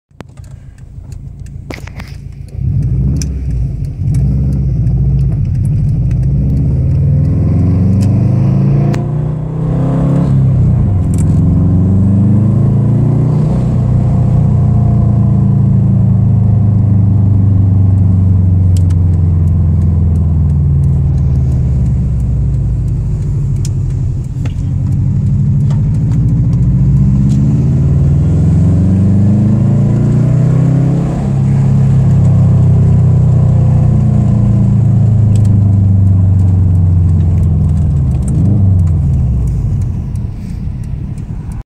2008 Subaru WRX EJ255 Slight Rod Knock Quick Drive
Subaru with rodknock, it's difficult to hear in this video. Great Subi boxxer sound, this is before pulling the engine out. EJ25. Baby TD04.